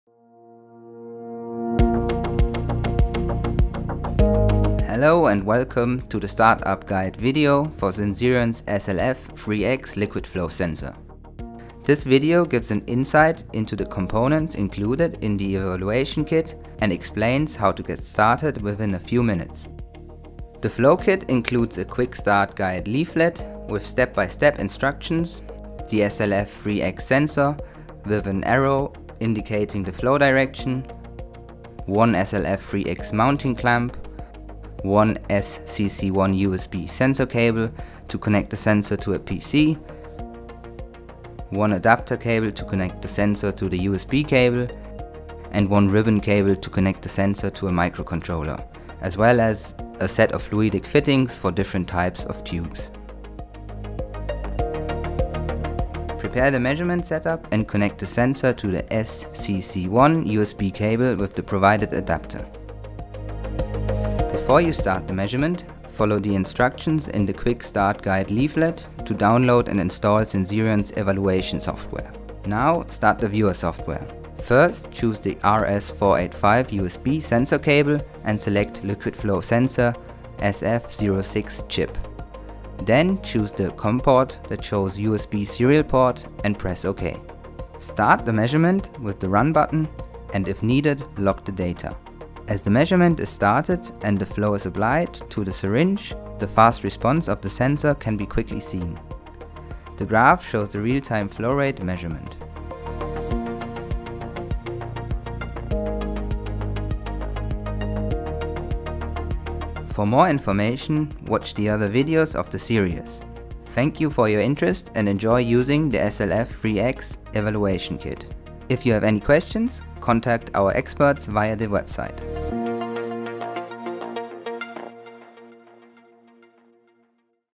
Sensirion Liquid Flow Sensors: Quick Start Guide
This video gives an insight on what components are included in the liquid flow evaluation kit and shows how to get started within a few minutes.

The liquid flow sensor SLF3S-1300F enables applications in the fields of diagnostics, analytical instruments and life sciences to reach unparalleled fluid control, system reliability and thus new heights in performance and end-user satisfaction. More information about the SLF3x liquid flow sensor